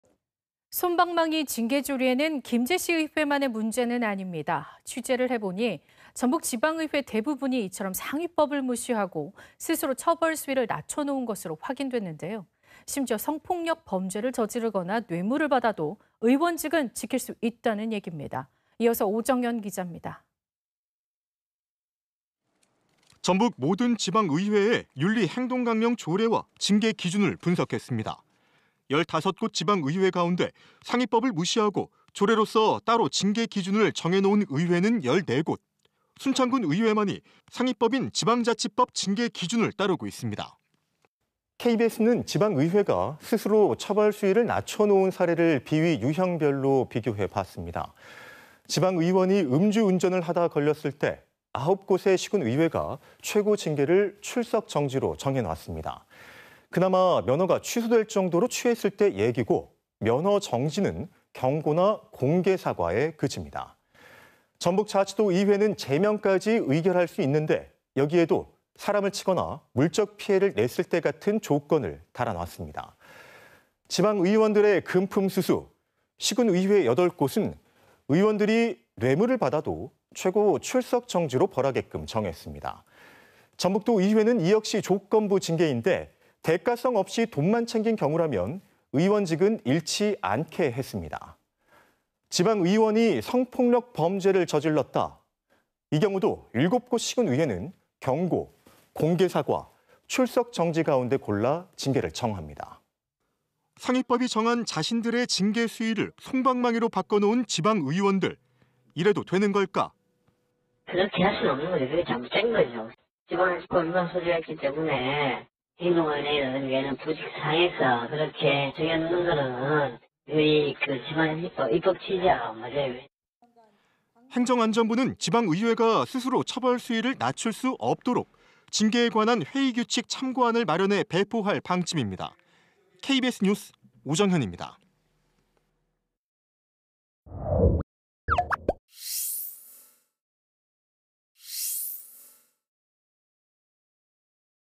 법도 무시하고 ‘솜방망이’…성폭력 징계가 고작 출석정지? / KBS 2024.04.23.
[앵커]

 '솜방망이' 징계 조례는 김제시의회만의 문제는 아닙니다.

 취재 결과, 전북 지방의회 대부분이 이처럼 상위법을 무시하고, 스스로 처벌 수위를 낮춰 놓은 것으로 확인됐습니다.

 심지어 성폭력 범죄를 저지르거나 뇌물을 받아도, 의원직은 지킬 수 있다는 얘기입니다.

 이어서 오정현 기자입니다.

 [리포트]

 전북 모든 지방의회의 윤리·행동강령 조례와 징계 기준을 분석했습니다.

 15곳 지방의회 가운데 상위법을 무시하고 조례로써 따로 징계 기준을 정해 놓은 의회는 14곳, 순창군의회만이 상위법인 지방자치법 징계 기준을 따르고 있습니다.

 KBS는 지방의회가 스스로 처벌 수위를 낮춰 놓은 사례를 비위 유형별로 비교해봤습니다.

 지방의원이 음주운전을 하다 걸렸을 때, 9곳의 시군의회가 최고 징계를 출석정지로 정해놨습니다.

 그나마 면허가 취소될 정도로 취했을 때 얘기고, 면허정지는 경고나 공개사과에 그칩니다.

 전북자치도의회는 제명까지 의결할 수 있는데, 여기에도 사람을 치거나 물적 피해를 냈을 때 같은 조건을 달아놨습니다.

 지방의원들의 금품수수, 시군의회 8곳은 의원들이 뇌물을 받아도 최고 출석정지로 벌하게끔 정했습니다.

 전북도의회는 이 역시  조건부 징계인데, 대가성 없이 돈만 챙긴 경우라면 의원직은 잃지 않게 했습니다.

 지방의원이 성폭력 범죄를 저질렀다?

 이 경우도 7곳 시군의회는  경고, 공개사과, 출석정지  가운데 골라 징계를 정합니다.

 상위법이 정한 자신들의 징계 수위를 솜방망이로 바꿔놓은  지방의원들, 이래도 되는 걸까?

 [행정안전부 관계자/음성변조 :  "그렇게 할 수는 없는 거죠. 잘못된 거죠, 지방자치법 위반 소지가 있기 때문에…. 행동강령이라든가 이런 부칙 상에서 그렇게 (징계 기준을) 정해 놓는 거는 우리 지방자치법 입법 취지와 안 맞아요."]

 행정안전부는  지방의회가 스스로 처벌 수위를 낮출 수 없도록 '징계에 관한 회의 규칙 참고안'을 마련해 배포할 방침입니다.

 KBS 뉴스 오정현입니다.

 촬영기자:안광석